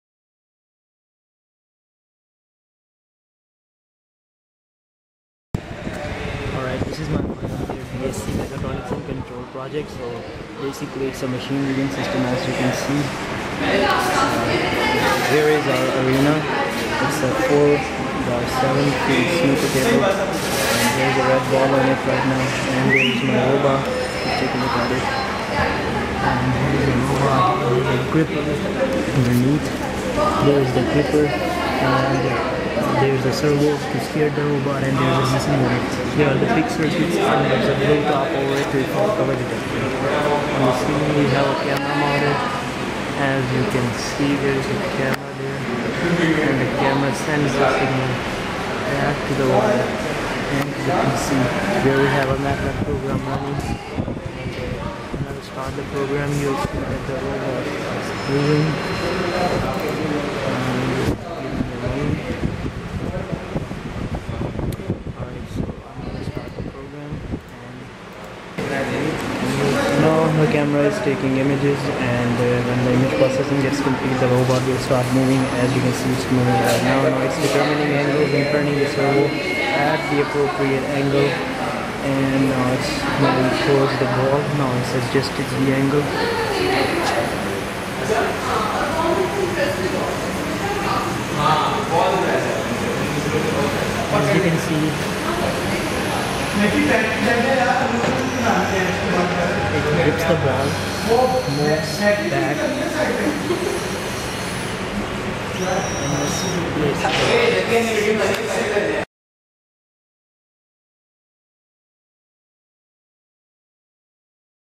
Snooker Table Arranging Robot aka S.T.A.R
This is my BSc Mechatronics & Control Final Year Project
The purpose was to arrange a pool table by using a machine vision system which included a camera mounted on the ceiling and a mobile robot. Though the project is not completed in all respects yet it was a great learning experience and with some work can do alot more